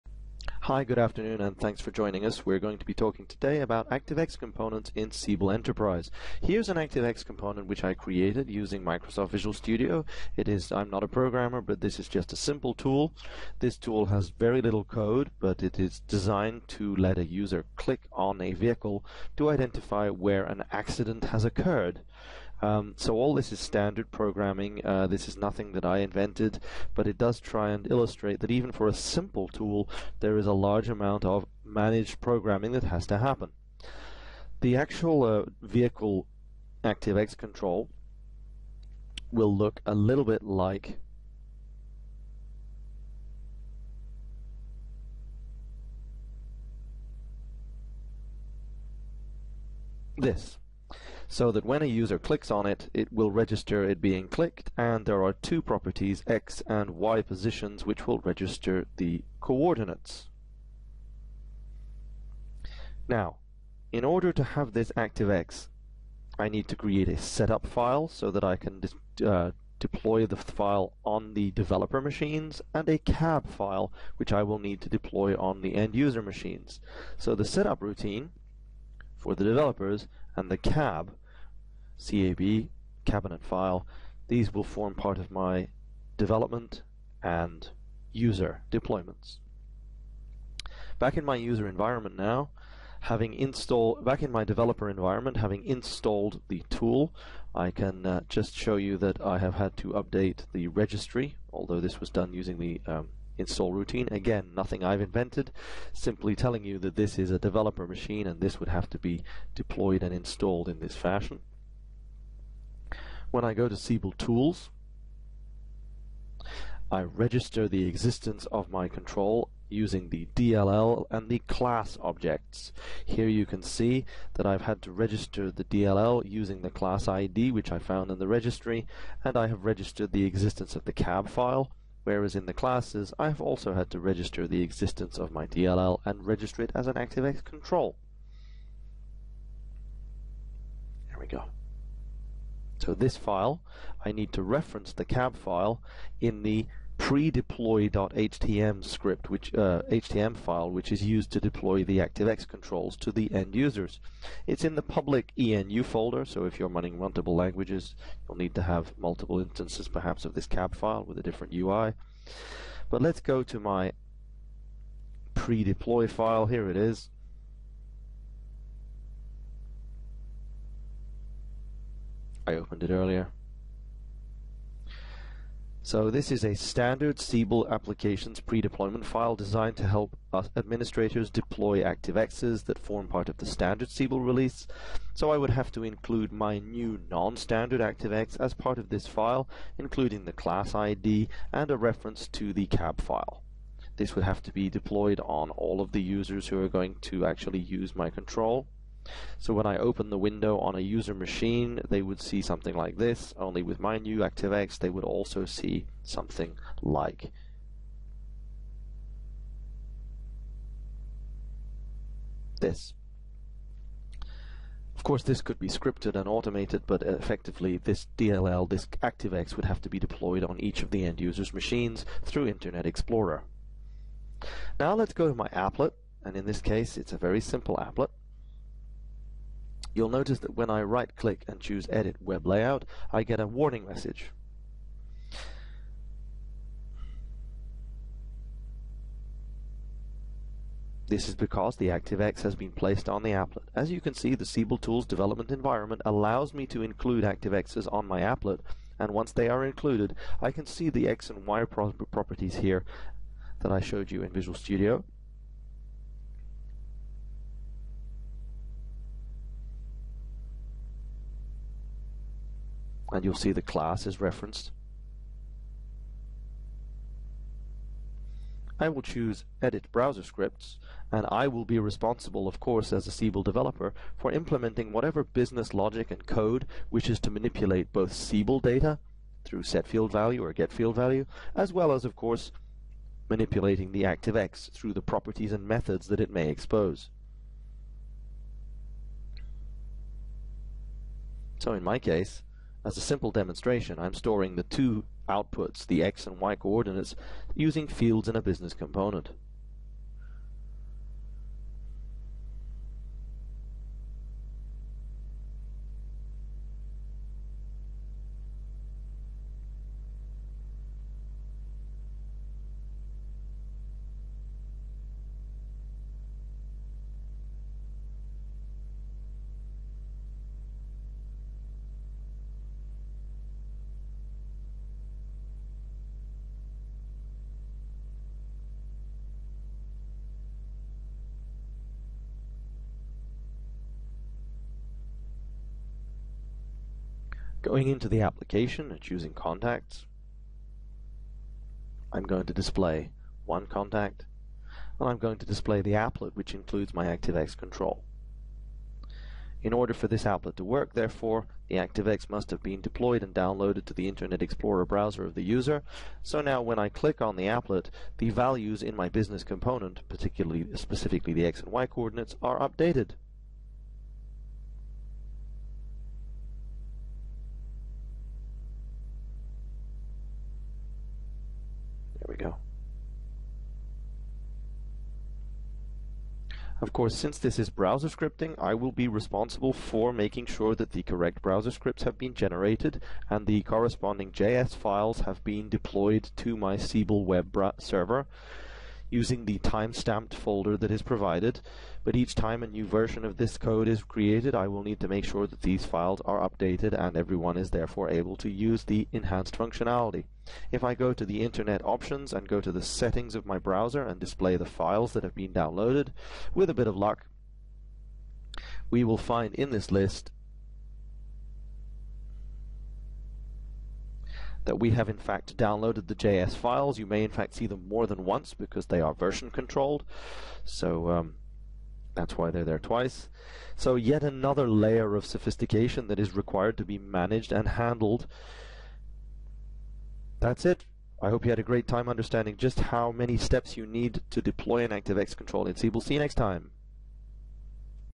Siebel Enterprise - adding new ActiveX
Flyby Video explaining how to add a custom ActiveX to Siebel Enterprise High Interactivity to support custom functionality using Siebel Tools and a Visual IDE of your choice such as MS Visual Studio, this example uses Siebel 8.1 and MS Visual Studio 2010, with an ActiveX deployed to Siebel Public Sector ENU